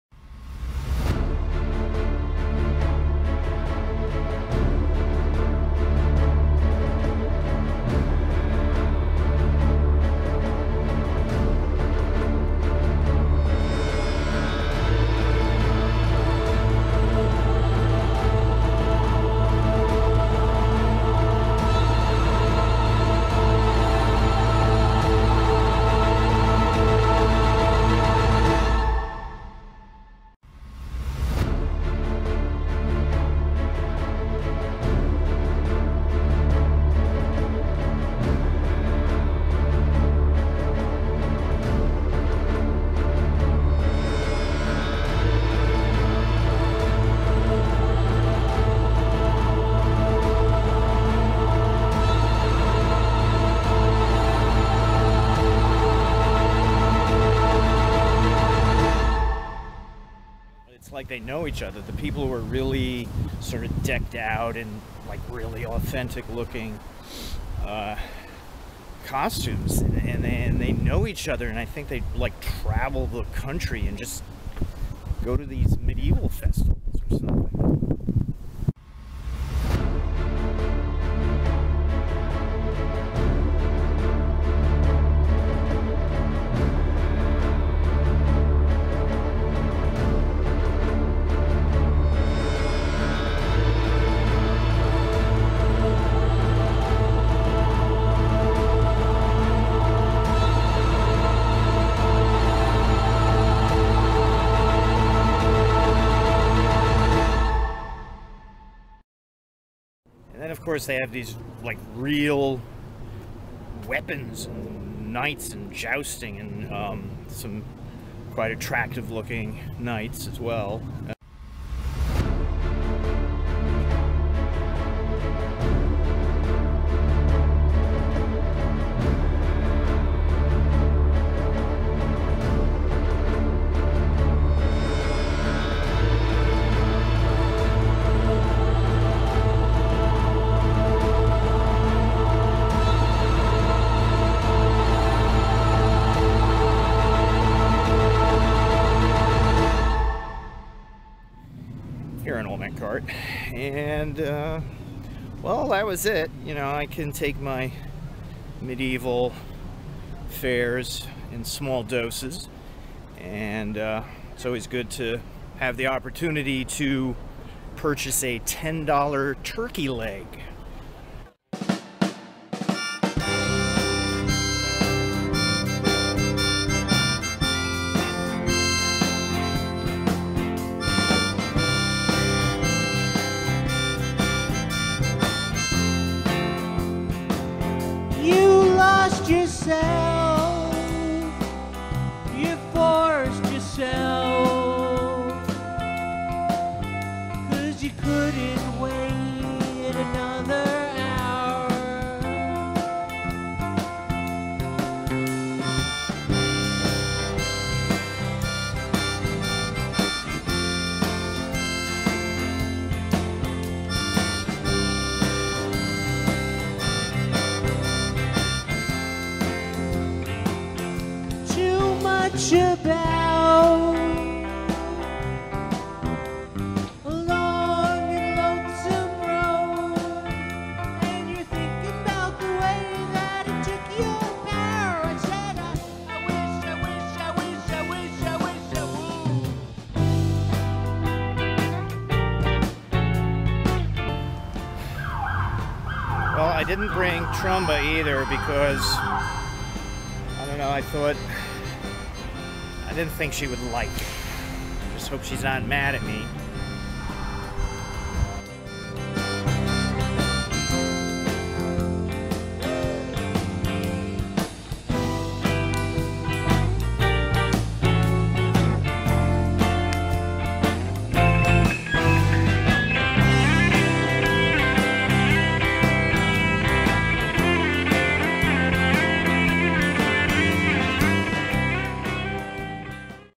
OMC Fort Tryon Medieval Festival 2015
The Medieval Festival is the busiest day of the year for Fort Tryon Park.  Certainly, as a setting, this beats any venue in the United States.  Come and get your $10 Turkey Drumsticks!